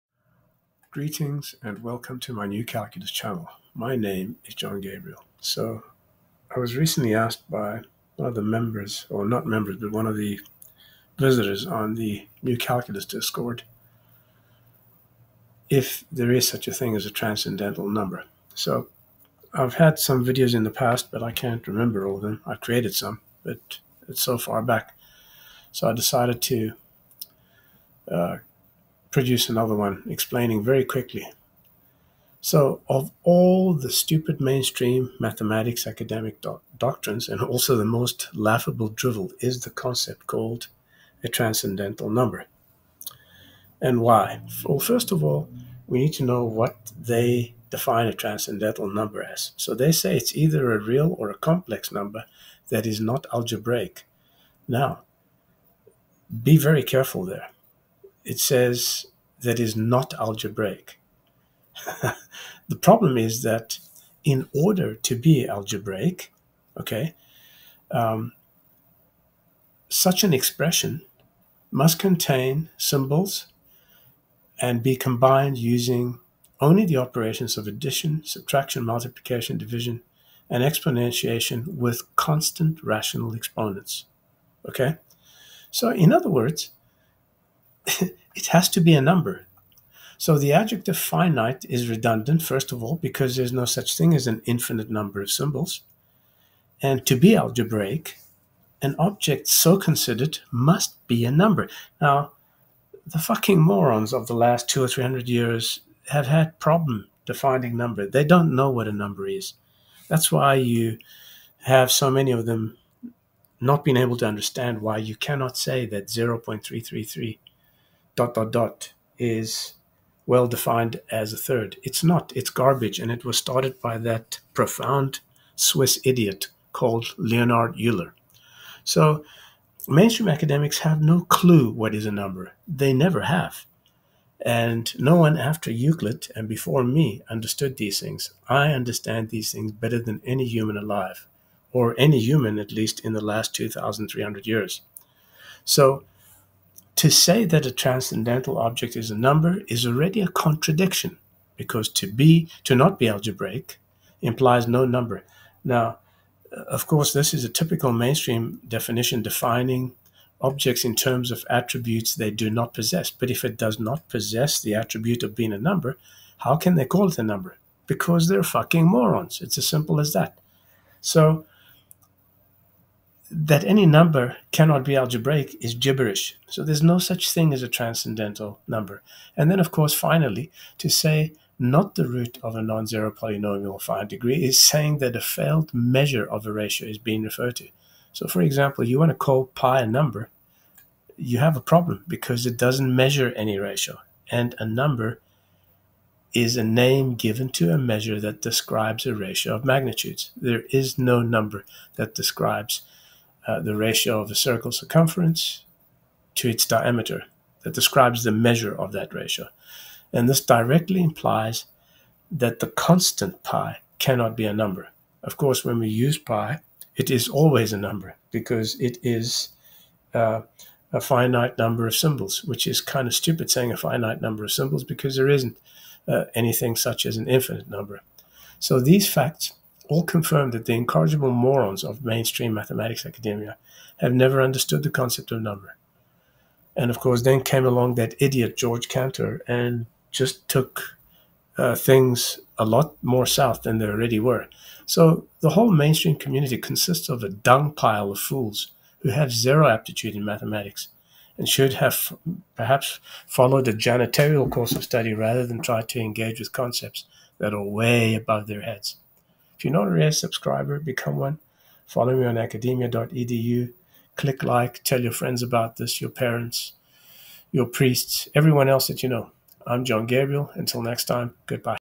No such thing as a transcendental number, only idiots who have never understood the number concept.
Mainstream mathematics community has no clue what it means to be a number, never mind the utter drivel called "transcendental number'. The idiots who coined this phrase were not only ignorant and incorrigibly stupid, but were probably also high on drugs.

Learn about the Gabriel Polynomial (GP) which is a very advanced concept not possible using mainstream derivative or integral definitions. The GP is always a closed form polynomial and has no Error term:

The scum of YouTube are constantly threatening and looking for excuses to shut down my channel.